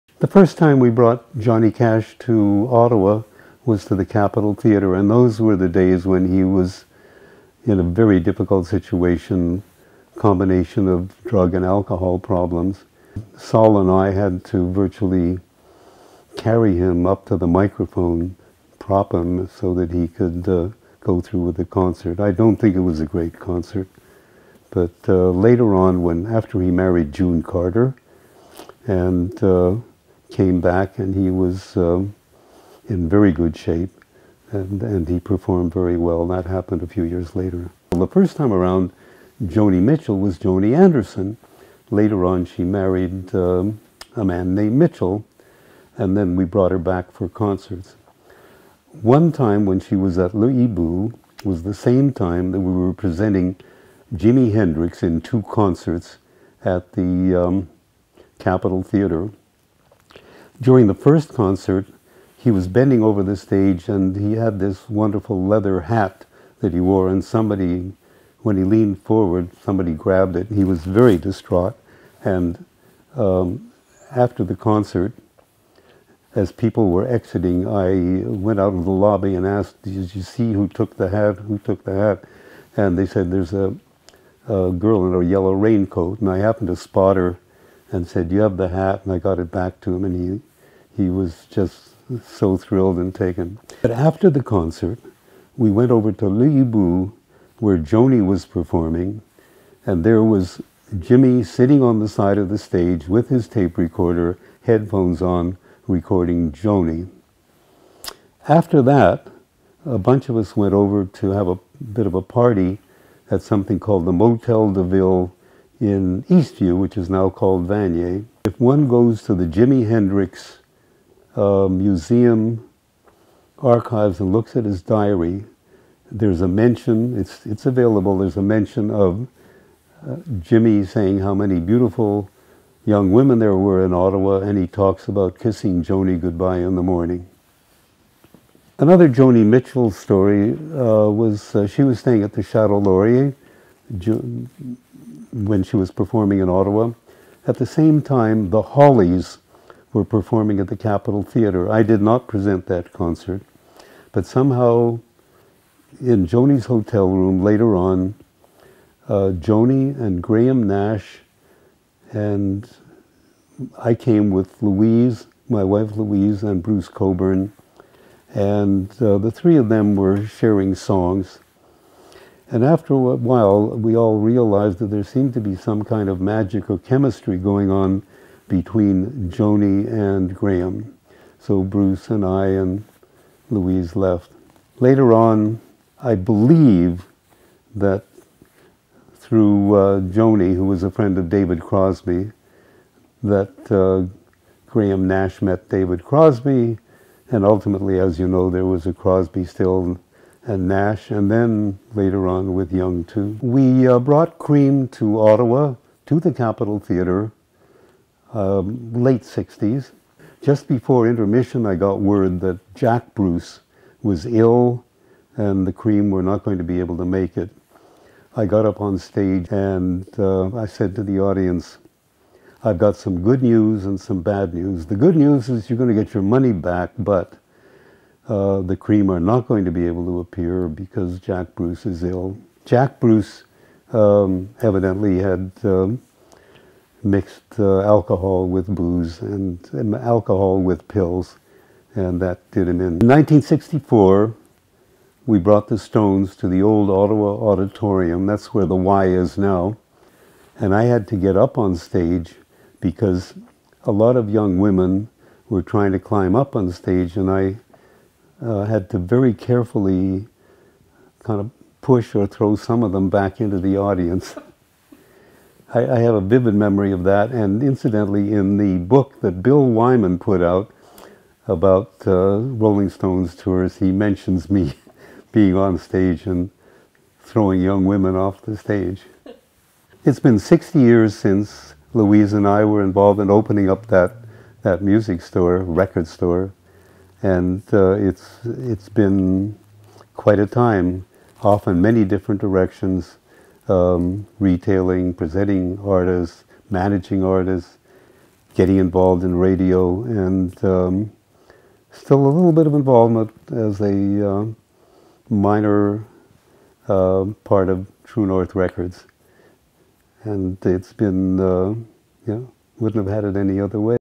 The Capital Buliders: Harvey Glatt opened Ottawa’s first real record store in 1957
Harvey Glatt opened Ottawa’s first real record store in 1957. Over the past 60 years (assuming this runs in 2017) he brought everyone from Johnny Cash and Joni Mitchell to Cream and The Rolling Stones to Ottawa  in addition to managing artists, publishing and owning Chez radio.